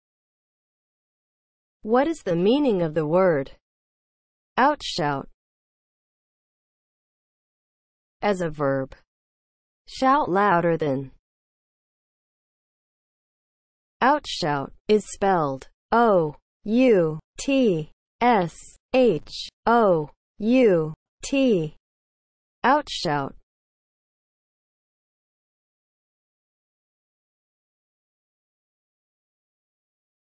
What is the meaning of the word OUTSHOUT?
Definition and spelling of the word OUTSHOUT.

(00:00) Intro
(00:07) As a verb
(00:12) Spelling

As a verb (00:07):
• shout louder than